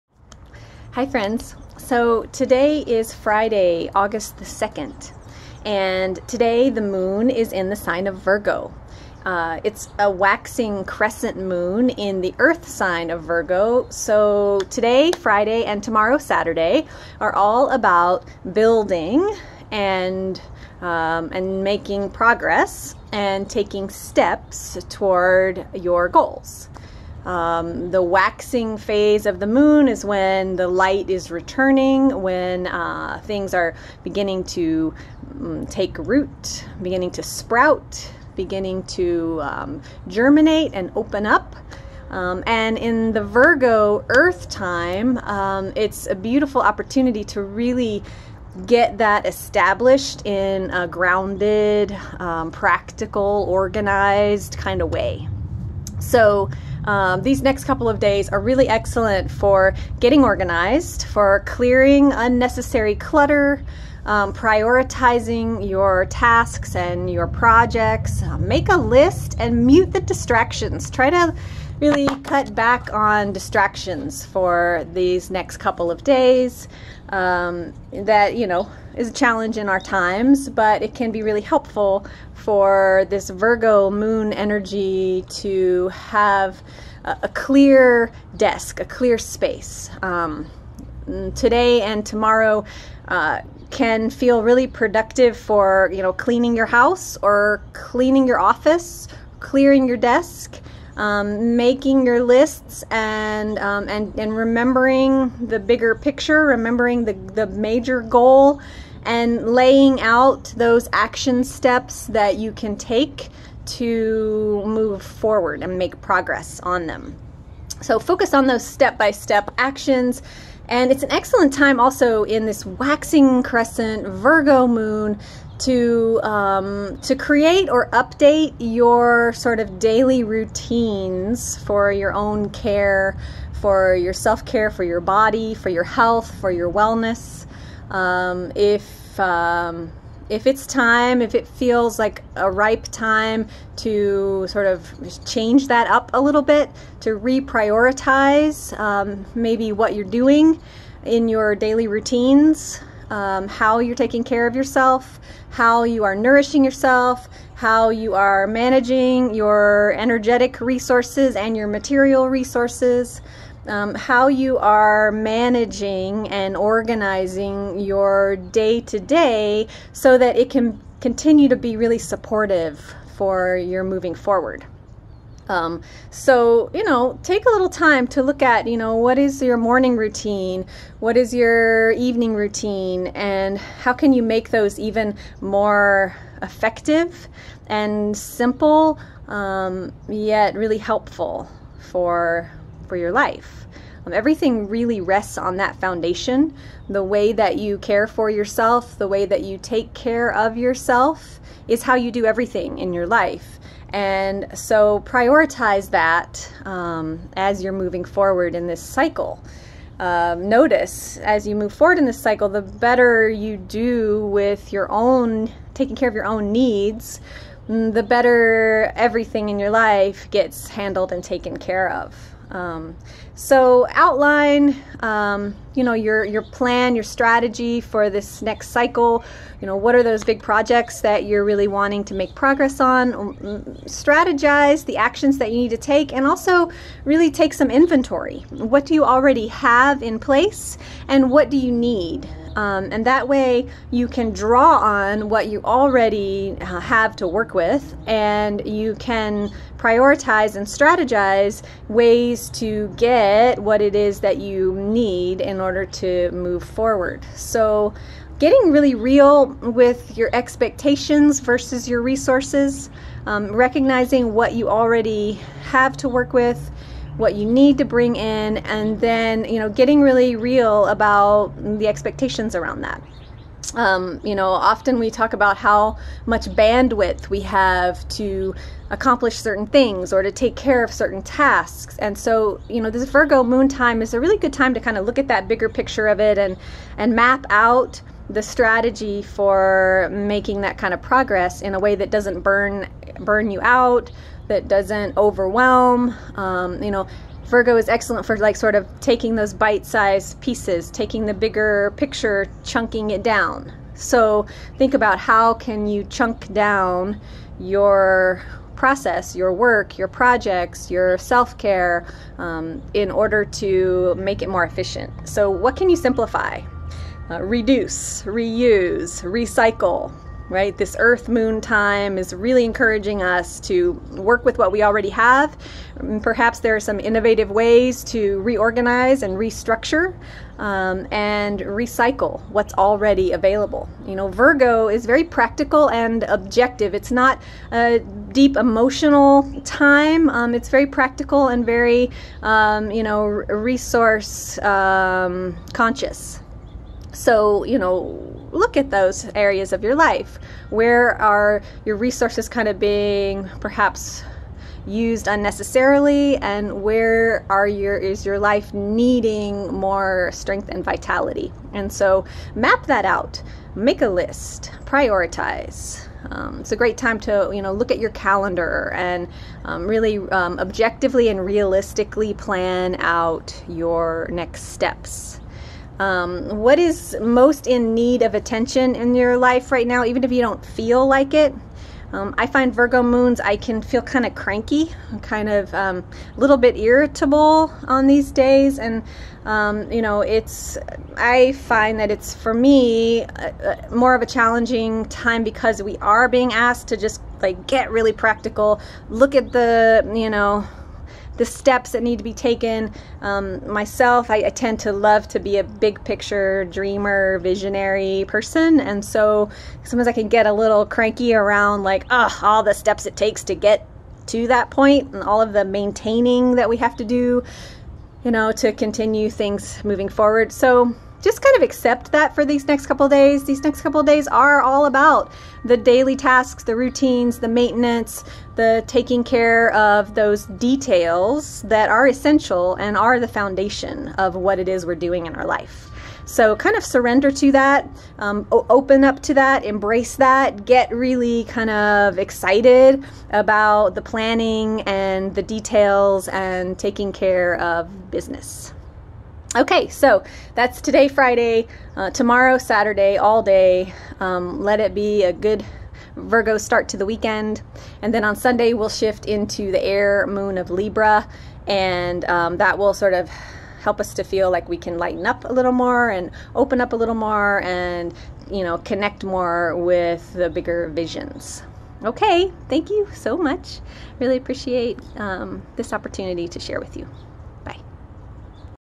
Virgo ♍ Earth 🌱 Moon 🌒 (waxing crescent) 8/2-8/3
Waxing crescent in Virgo invites us to clear away the clutter and distractions so we can get organized and prioritize our actions for moving forward. Create or update practical and reasonable self care routines. How you care for yourself is the foundation for how you do the rest of your life!